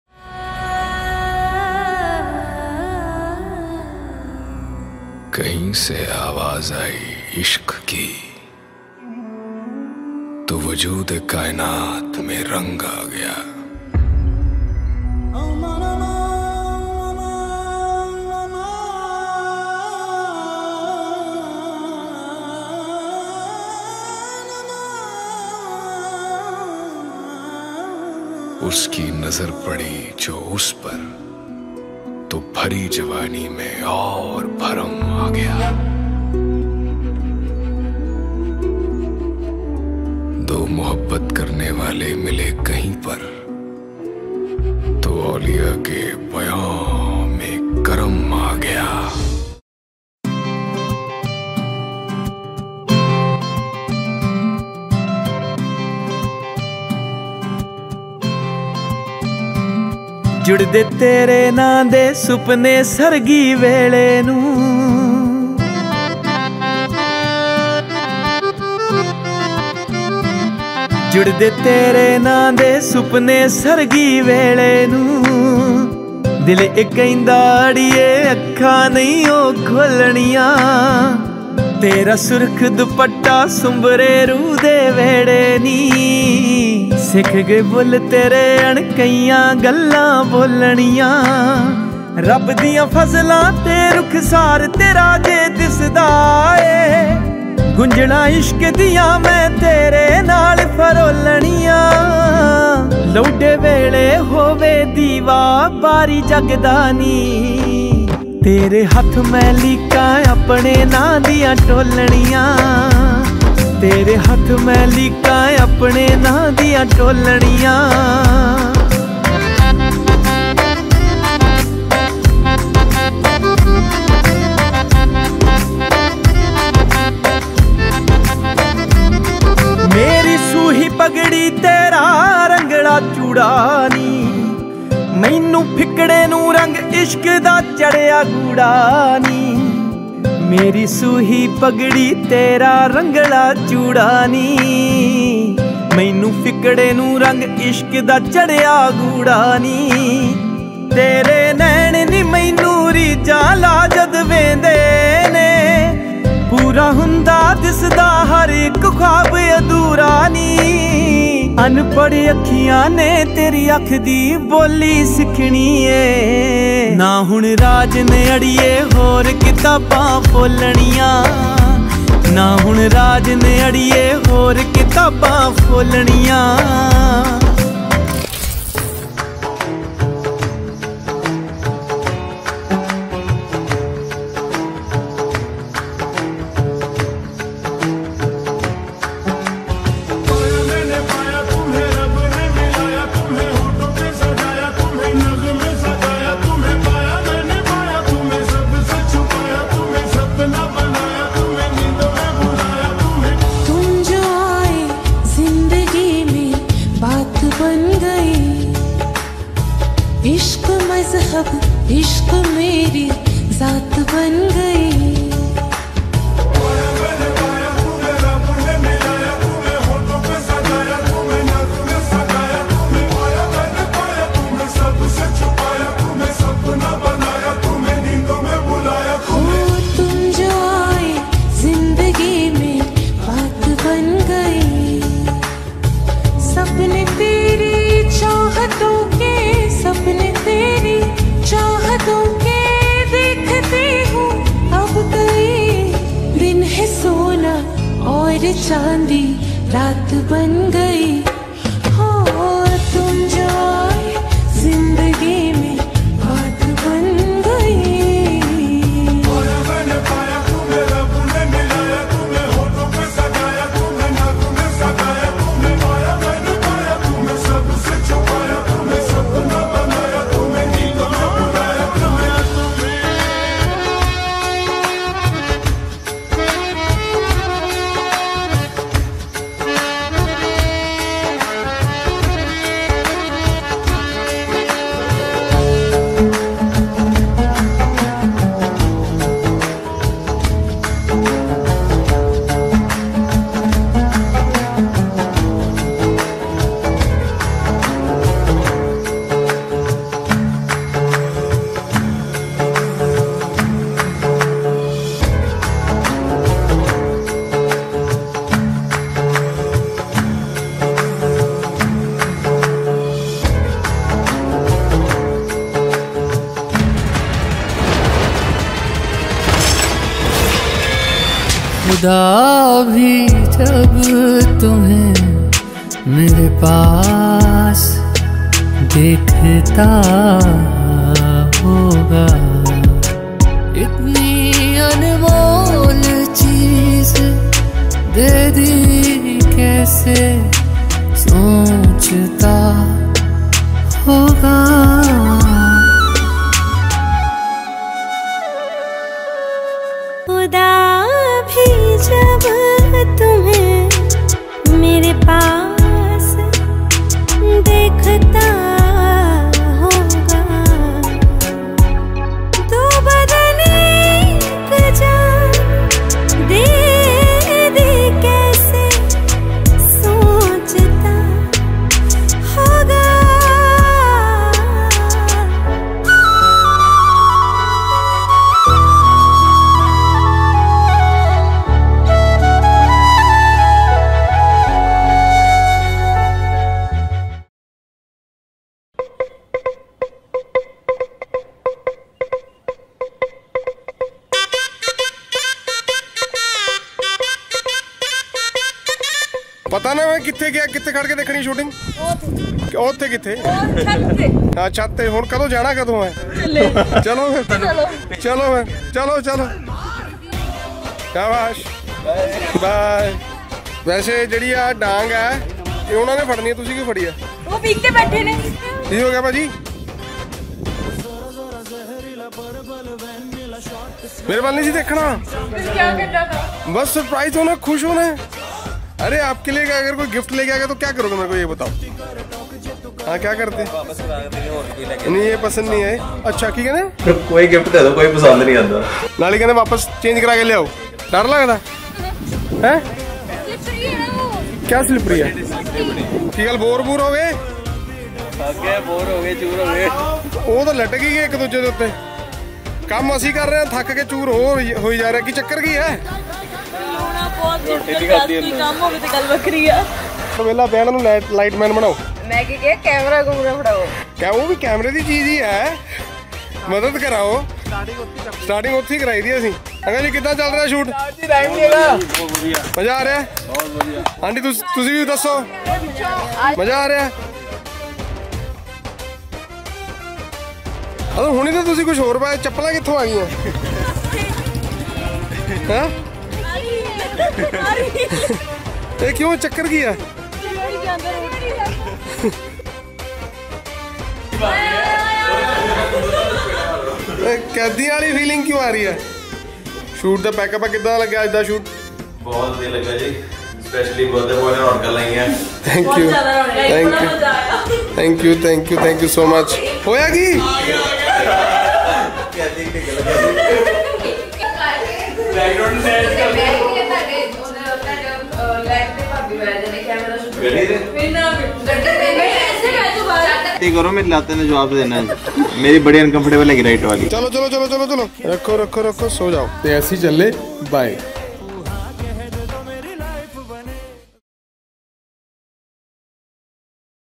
Best Pre-wedding (Jaskirat Singh and Anmolpreet Kaur)
Pre wedding of Jaskirat Singh and Anmolpreet Kaur at Lal Haveli Nabha with funny Bloopers and Behind The Scenes.